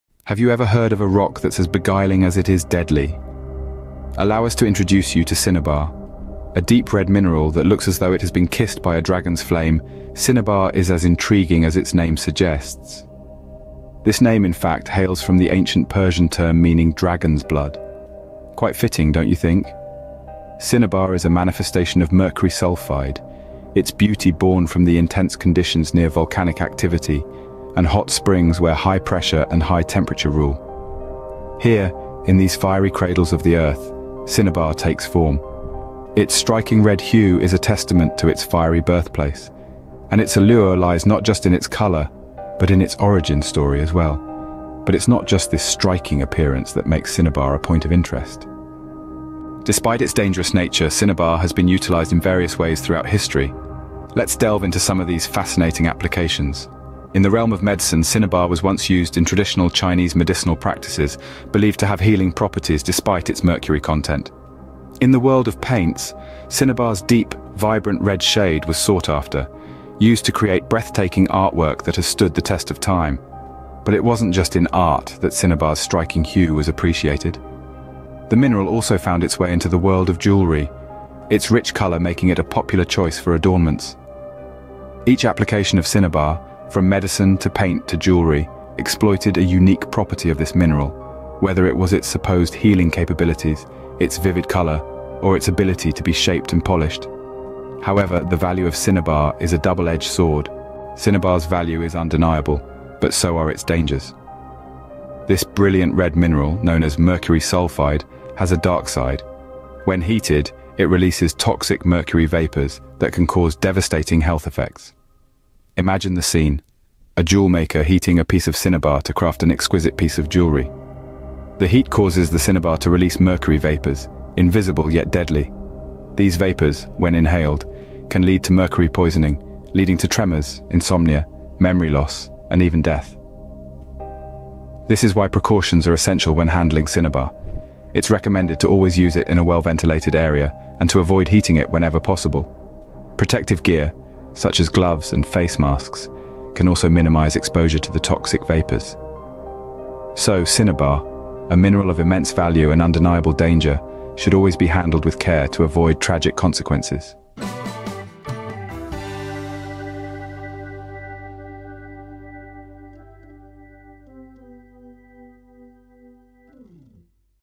Cinnabar: Precious and Dangerous
In this captivating video, join us as we delve into the world of Cinnabar, an intriguing mineral that holds both precious and dangerous qualities. From its stunning aesthetics to its historical significance, we'll explore the various aspects that make Cinnabar a true gem of the mineral world. But beware, we'll also uncover the potential hazards associated with this mercury sulfide mineral. So sit back, relax, and prepare to be mesmerized by the beauty and complexity of Cinnabar.

OUTLINE:

00:00:00 The Mysterious and Dangerous Cinnabar
00:00:49 The Many Faces of Cinnabar
00:01:40 The Value and Dangers of Cinnabar